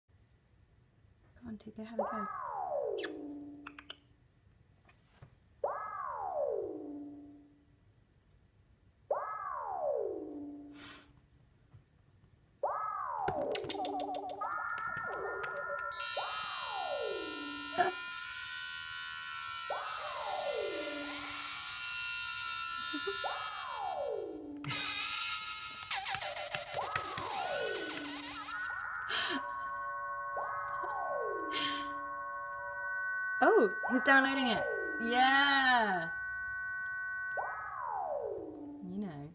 Tuco the cat aka Theremin synth master
Yes, this is a video of a cat jamming on a Theremin app.